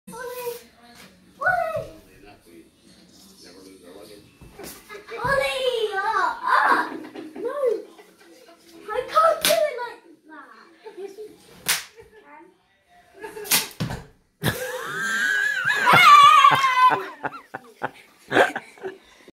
Kung Fu Kick GONE WRONG
Soldier Charlie practicing karate